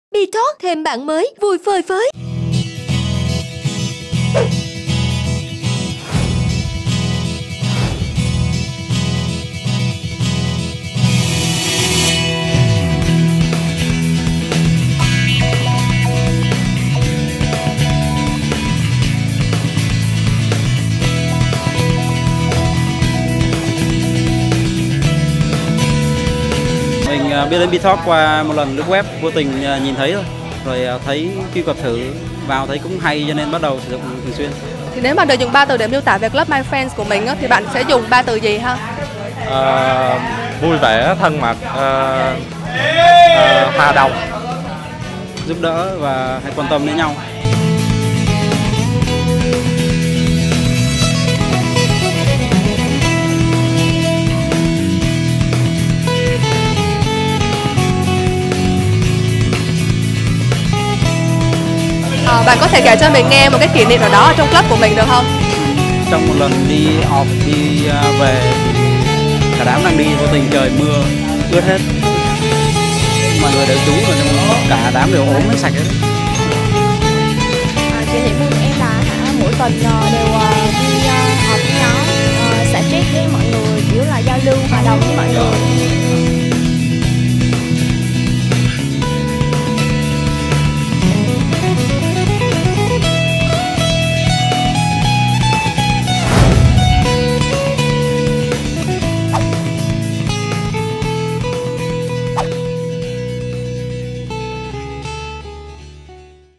BeeTalk: "Vòng quay may mắn" đợt 4 - Đồng Nai
Tưng bừng chuỗi sự kiện "Vòng quay may mắn - NÀO TA CÙNG TÁM", BeeTalk đã ghé Đồng Nai vào 1 ngày cuối năm nè! Cùng theo chân buổi offline thú vị của BeeTalk cùng Club "My Friends" nhé.